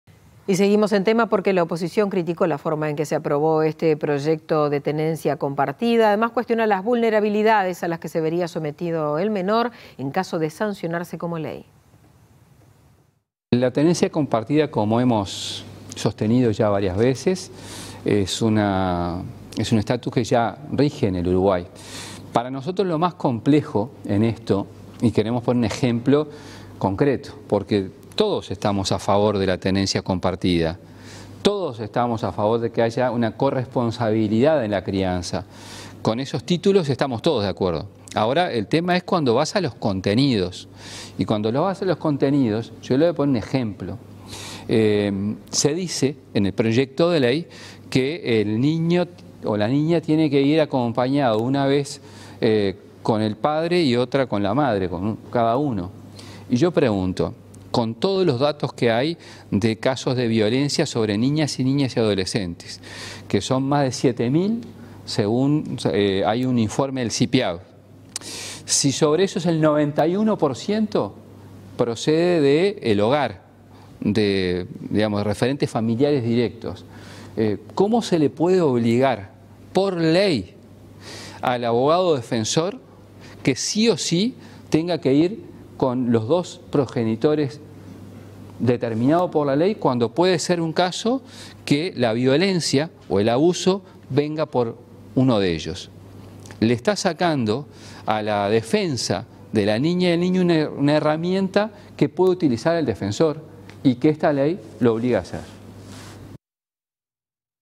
La oposición criticó la forma en que se aprobó el proyecto de ley de Tenencia Compartida
El senador José Carlos Mahía cuestionó las vulnerabilidades a las que se vería sometido el menor, en caso de sancionarse como ley.